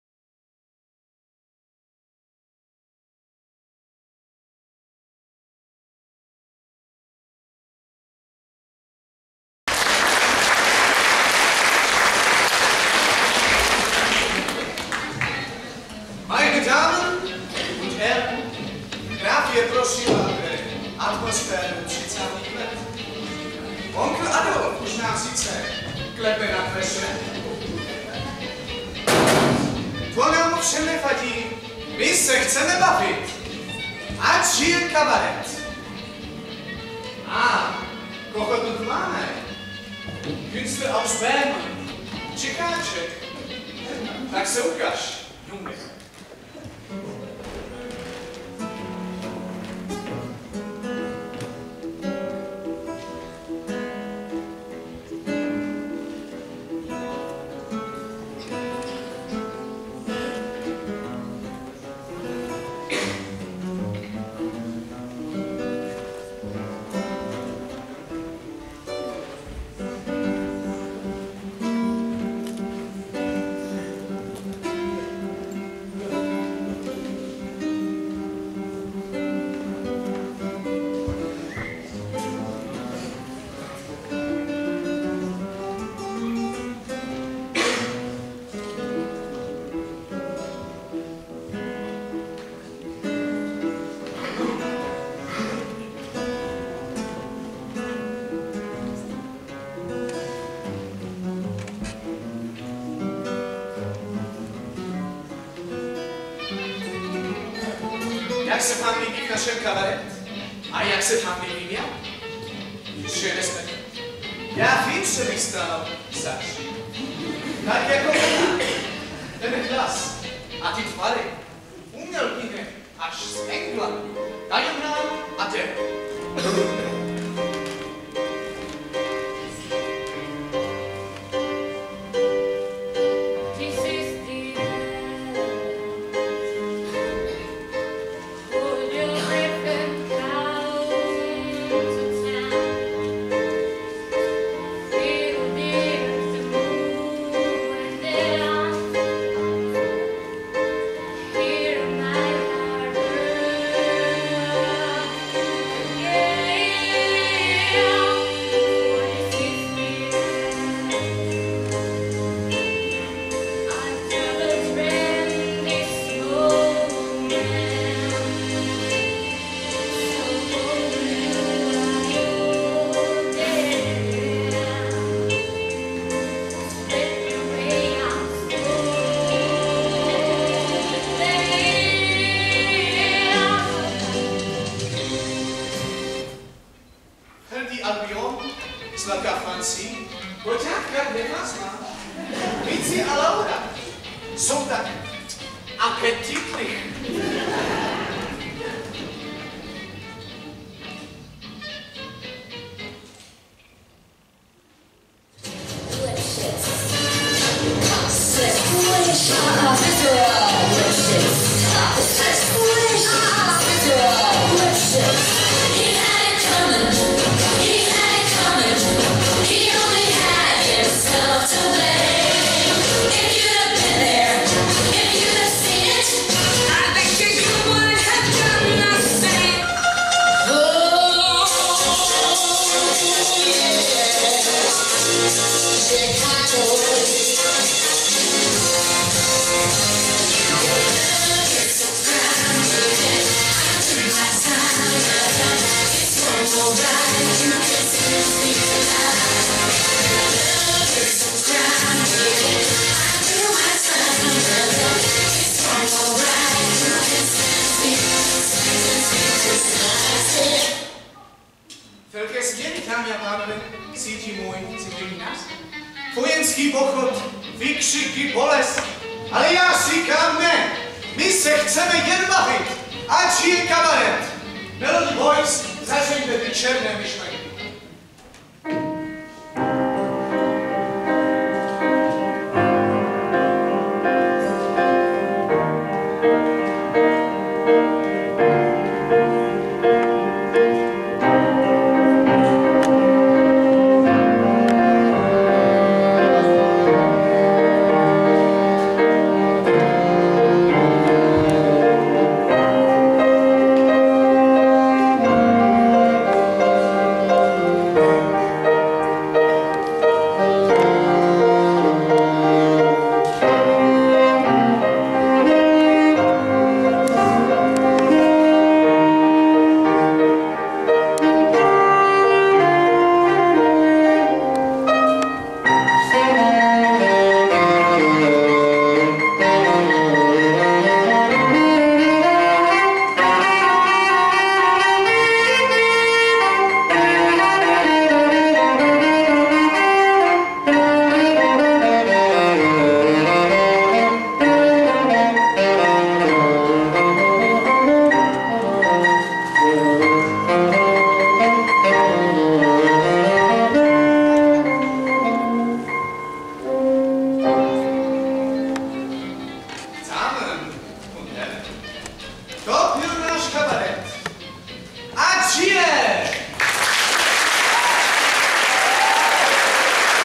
Střední průmyslová škola - Kabaret
Vystoupení Střední průmyslová škola strojní, stavební a dopravní Děčín v podobě večerního vystoupení s názvem Kabaret.